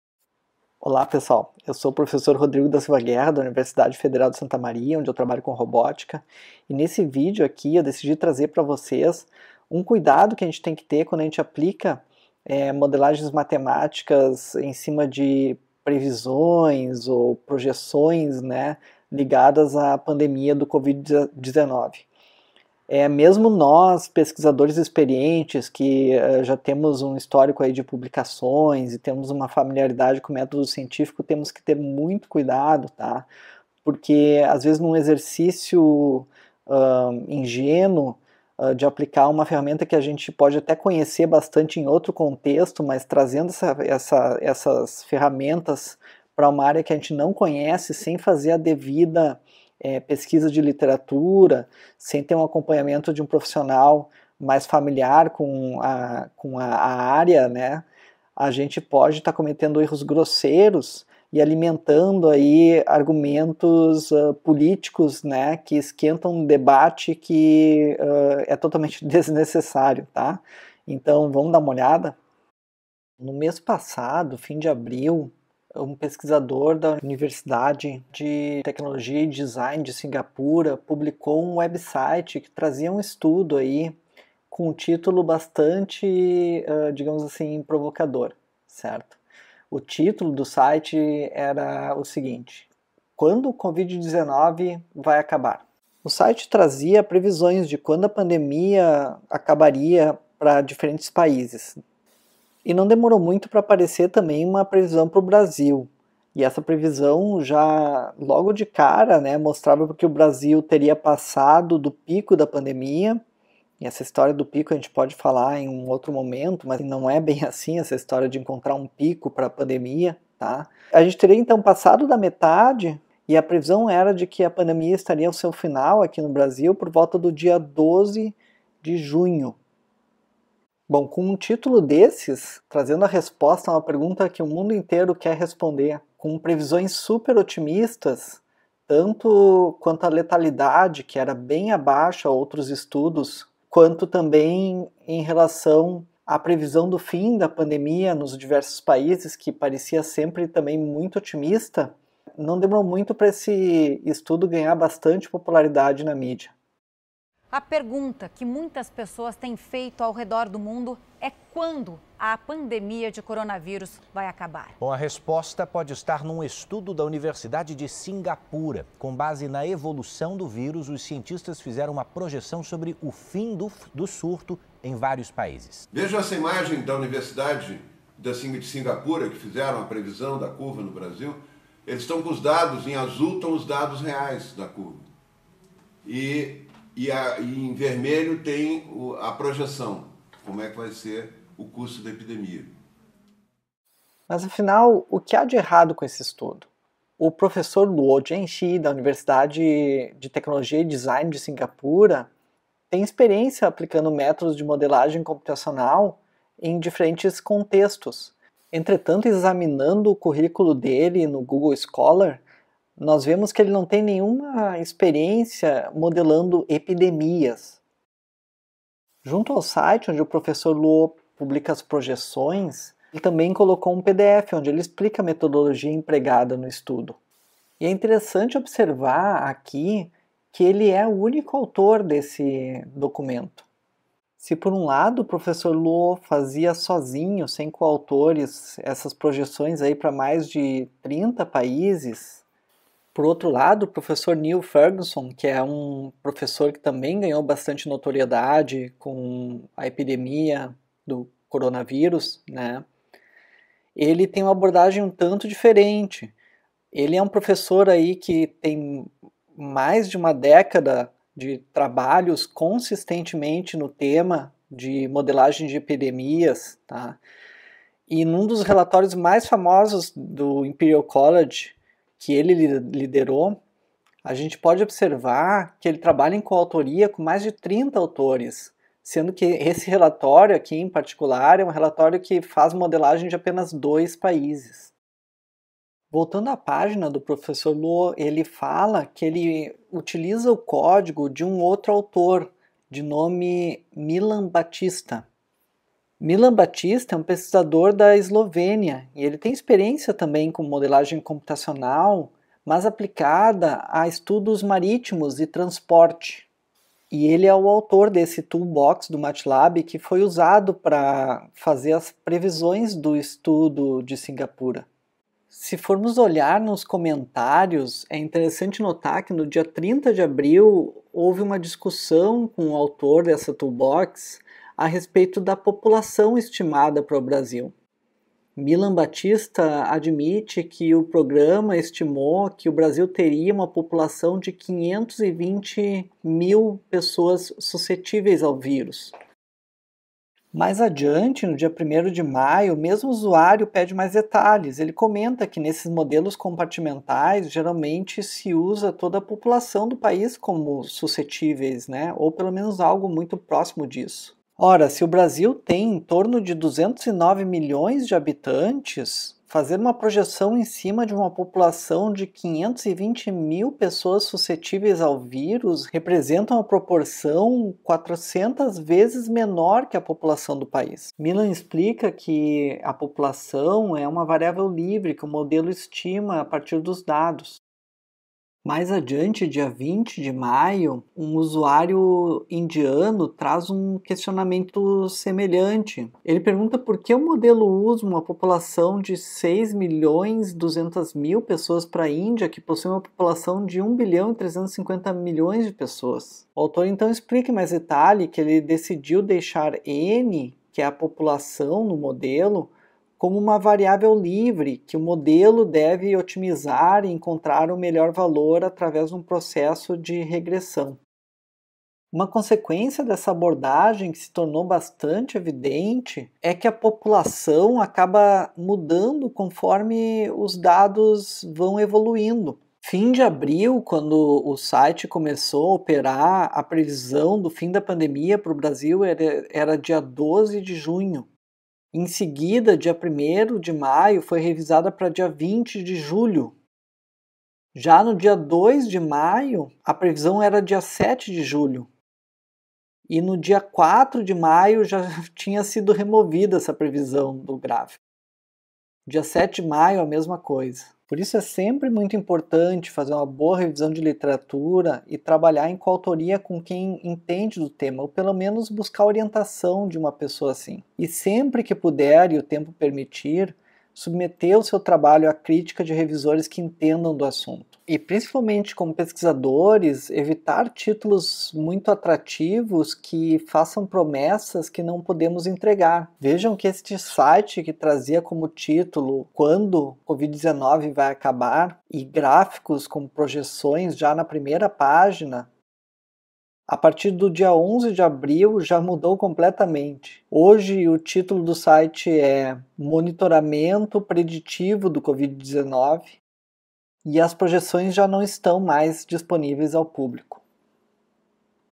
Sobre o Estudo de Singapura
Neste vídeo trago algumas reflexões sobre o famoso estudo de Singapura, que prometia responder "Quando COVID-19 vai acabar". Como pesquisador me identifico com os colegas do referido estudo que tinham a boa intenção de ajudar, aplicando técnicas que dominam, mas numa área que não dominam. Aplicaram um método "a rodo" para dezenas de países do mundo, sem olhar pormenores, e acabaram cometendo erros grosseiros. Se és pesquisador e queres ajudar no enfrentamento à pandemia, tome cuidado para revisar bem a literatura e busque um co-autor ou orientador que domine as áreas de conhecimento que são novidade para ti. Senão, tentando ajudar, podes acabar atrapalhando bastante! 😉👍